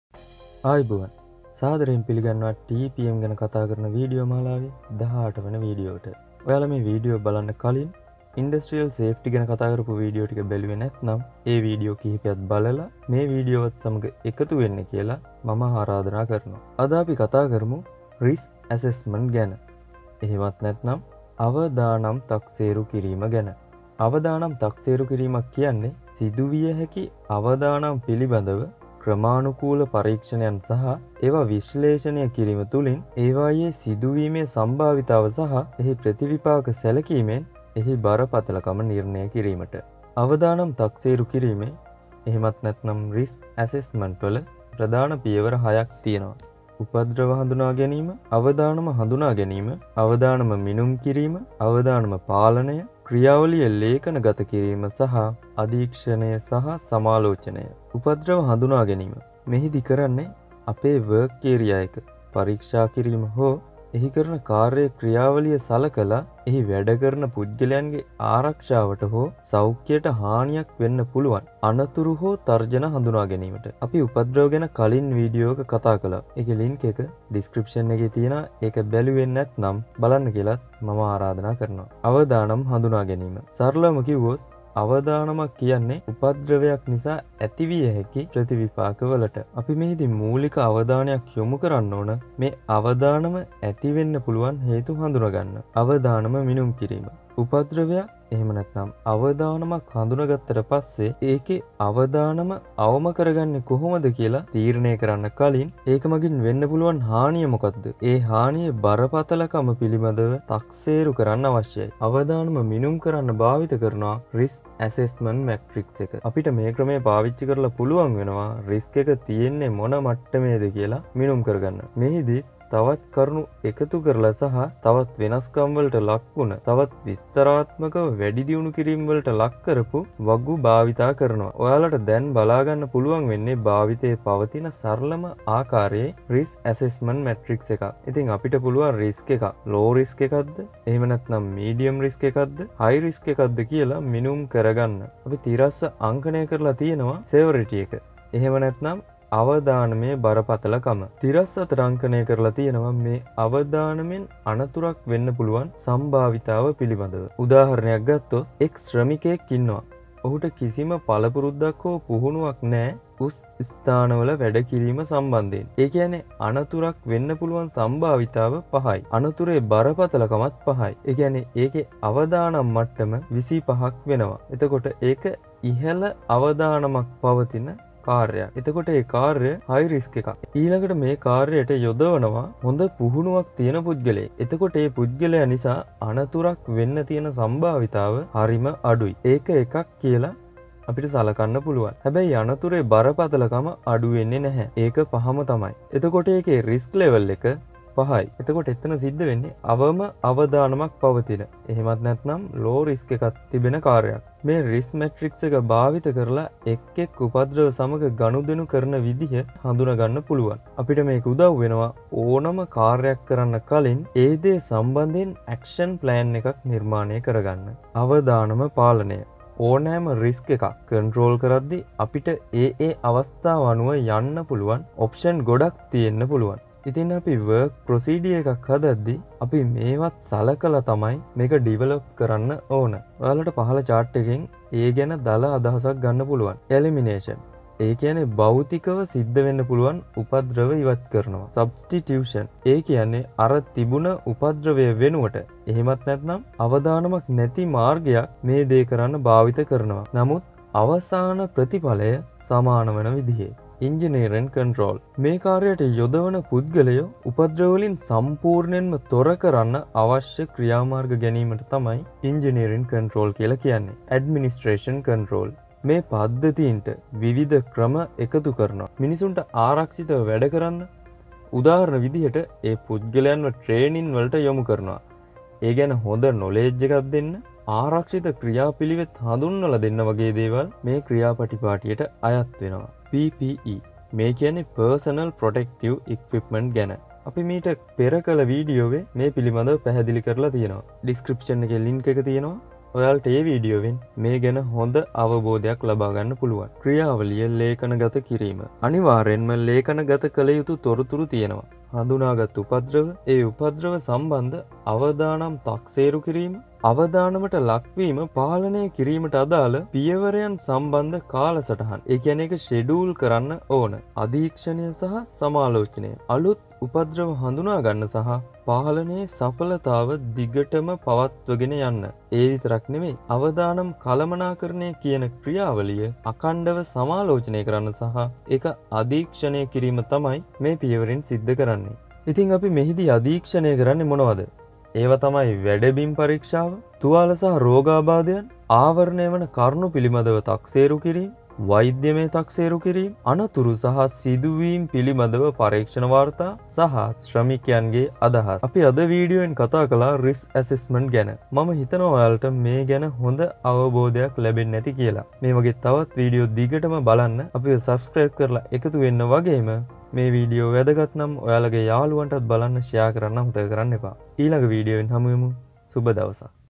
Risk Assessment | Total Productive Maintenance in Sinhala | Part 18
Risk Assessment
Total Productive Maintenance in Sinhala
Part 18

A risk assessment is a process in which businesses identify and assess the risks associated with their operations. This helps them to develop strategies to minimize or mitigate the risks.